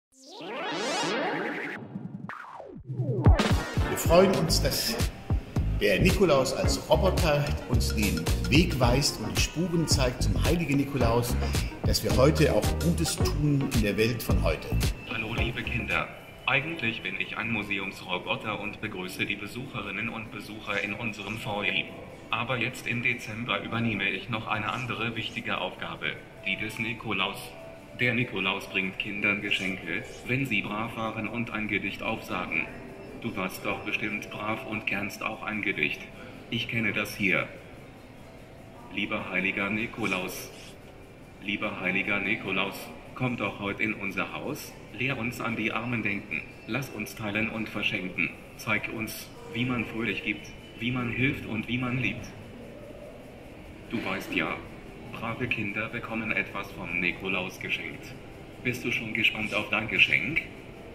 Der ganz besondere Nikolaus-Vertreter
Ein Roboter ist nichts Ungewöhnliches im weltgrößten Computermuseum. Doch ab dem 6. Dezember bis Weihnachten fährt ein Roboter im Foyer des Heinz Nixdorf MuseumsForums herum, der als Nikolaus gekleidet ist. Er begrüßt die kleinen sowie großen Besucher und informiert über das Leben des heiligen Nikolaus. Und wie es sich gehört, verteilt er auch Geschenke. Die Jüngsten können dem Roboter-Nikolaus folgen und erhalten eine süße Überraschung. Mit der Aktion unterstützt das HNF die "Weihnachtsmannfreie Zone" des Bonifatiuswerkes.